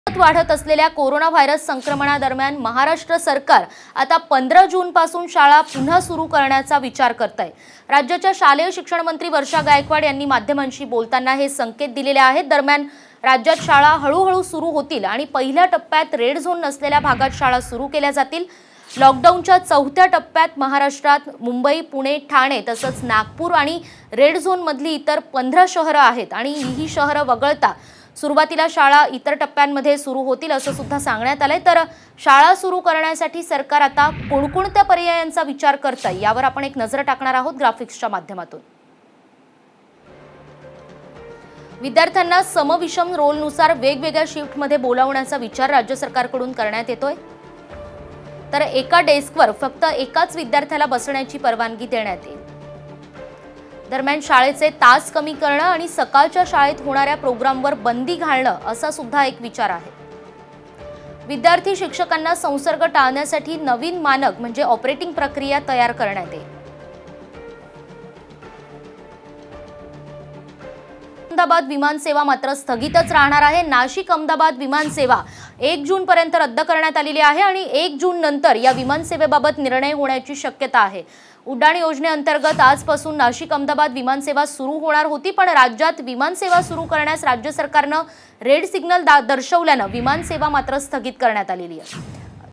BREAKING | राज्यात 15 जूनपासून शाळा सुरू होणार?
देशात सतत वाढत असलेल्या कोरोनाव्हायरस संक्रमणादरम्यान... महाराष्ट्र सरकार 15 जूनपासून शाळा पुन्हा सुरू करण्याचा विचार करतय... राज्याच्या शालेय शिक्षणमंत्री वर्षा गायकवाड यांनी माध्यमांशी बोलताना असे संकेत दिलेत.. राज्यात शाळा हळूहळू सुरू होतील आणि पहिल्या टप्प्यात रेड झोन नसलेल्या भागात शाळा सुरू केल्या जातील. लॉकडाऊनच्या चौथ्या टप्प्यात, महाराष्ट्रात मुंबई, पुणे, ठाणे, नागपूर आणि रेड झोनमधील अन्य 15 शहरे आहेत...